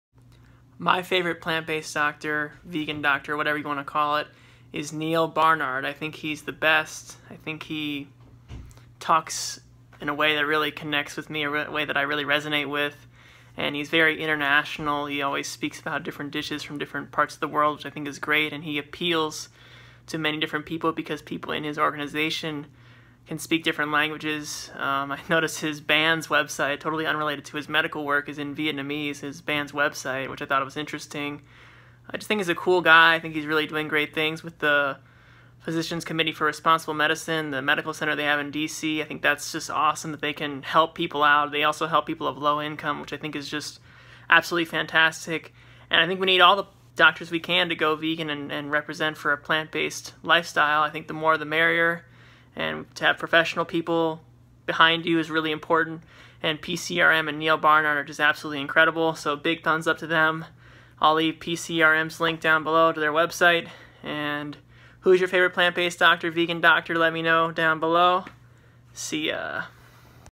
My Favorite Vegan Doctor
Today I talk about who my favorite vegan doctor is and why he's my favorite. This guy is changing lives and making the world a better place!

Who's your favorite vegan doctor?

Follow me on Strava: Alex Curtis-Slep

Go Neal Barnard!